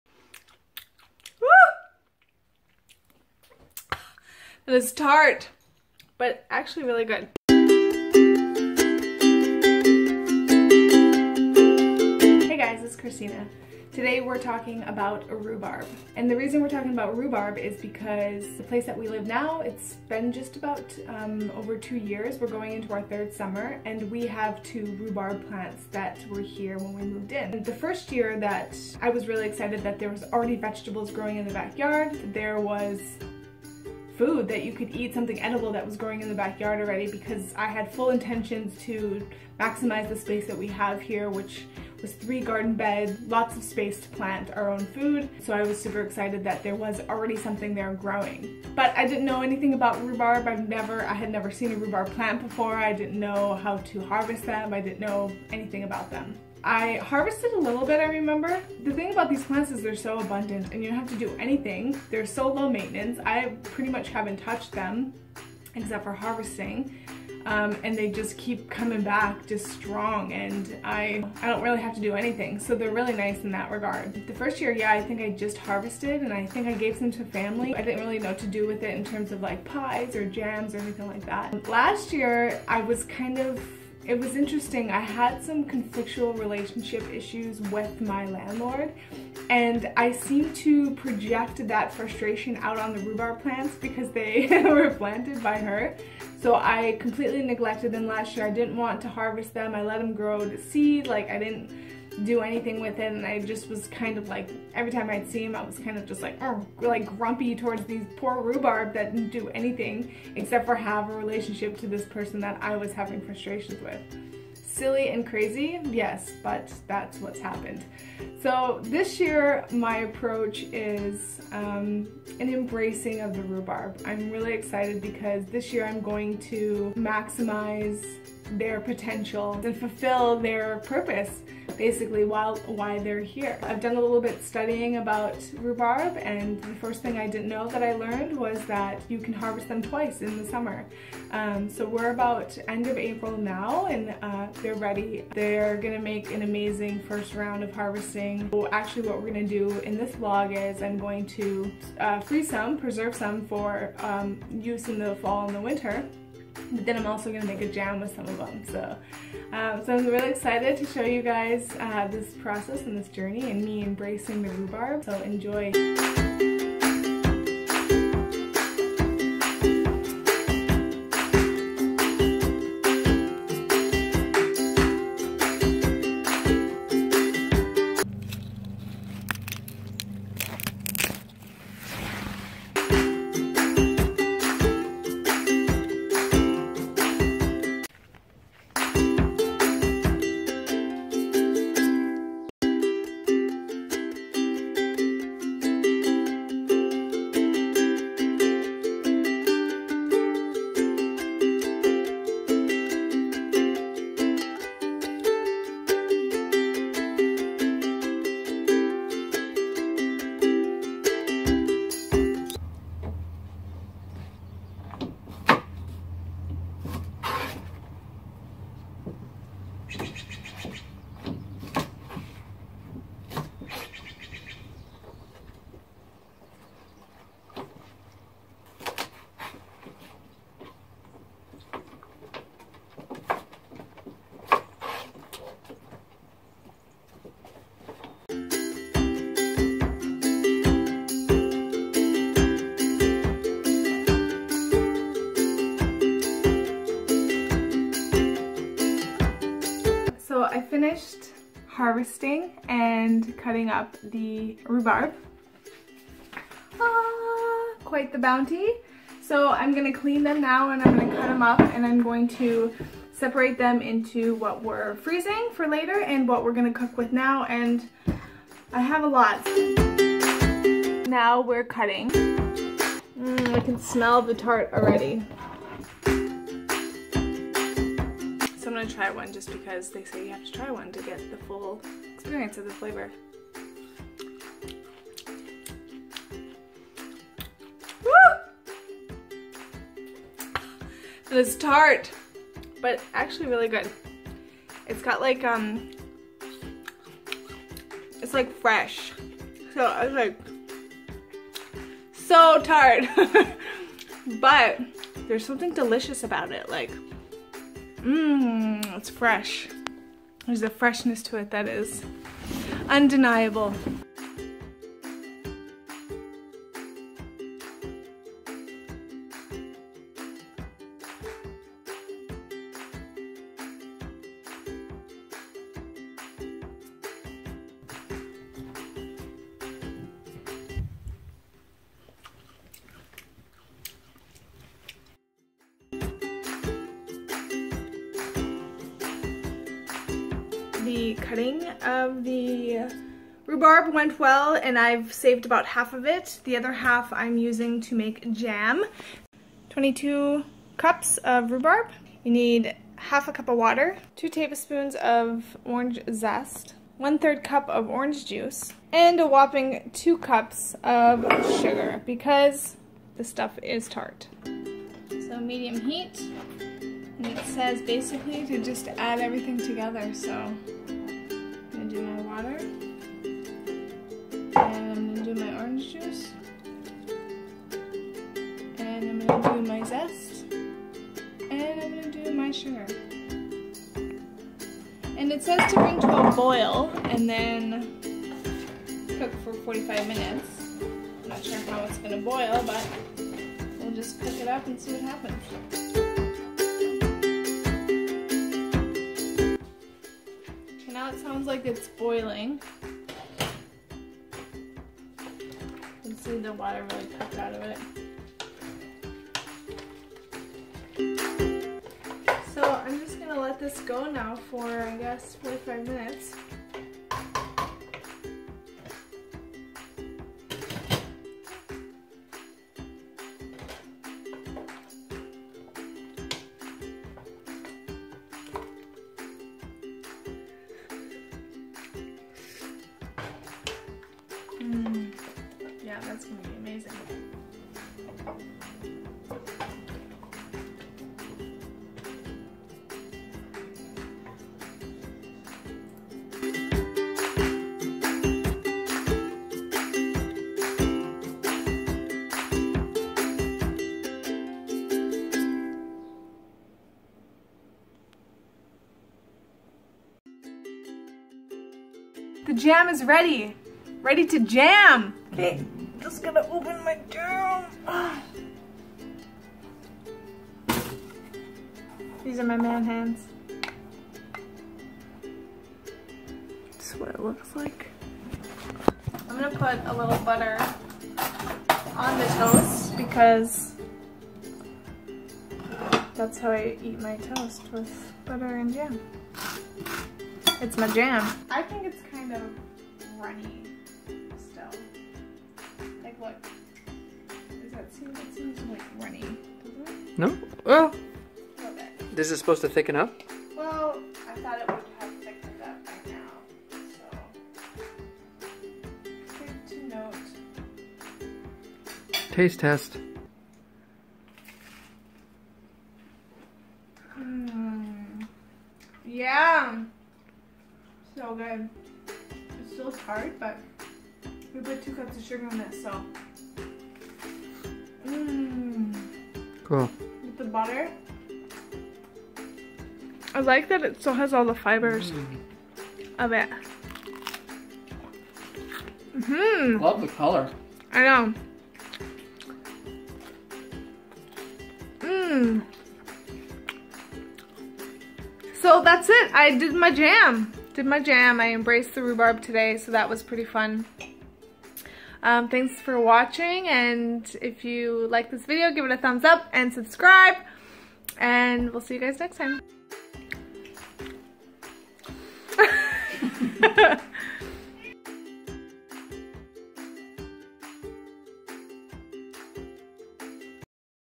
Embracing Rhubarb - Harvesting, Freezing, and Jammin'
I got two rhubarb plants and I'm utilizing their full potential! Harvesting, freezing for late summer or fall use and making some jam (recipe included)

This is my first time in all above-mentioned activities. Come join me on the journey.

Change yourself and the world: lite.desteniiprocess.com